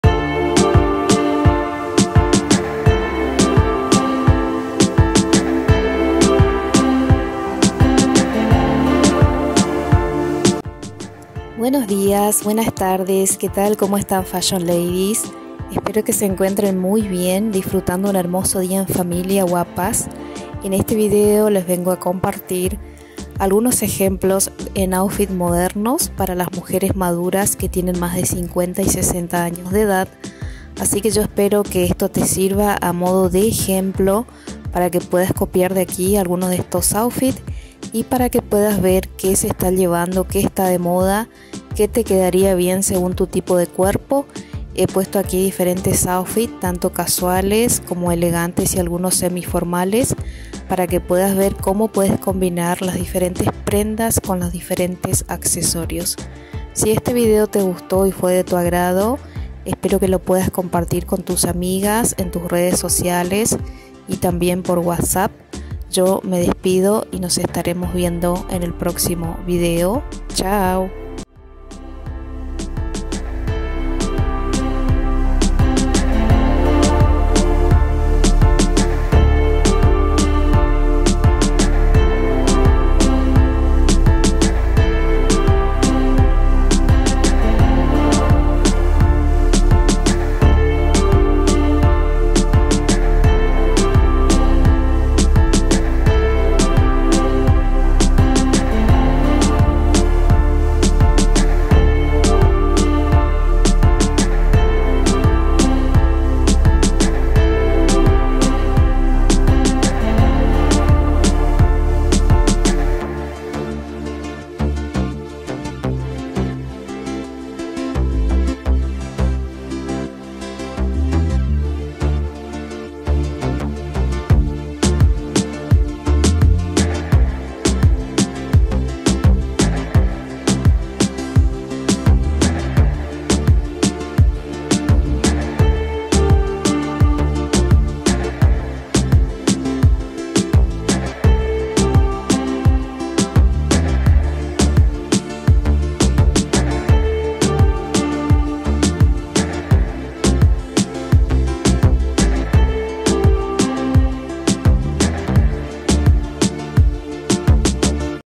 MODA PARA MUJERES MADURAS DE 50 60 Y 70 AÑOS ROPA PARA SEÑORAS DE 50 60 Y 70 AÑOS
Bienvenidas Fashion Ladies señoras de la moda en esta ocasión vamos a ver una colección en ropa moderna para las señoras y mujeres maduras que gustan vestir a la moda combinando prendas de tenencias pero usando la ropa según la edad .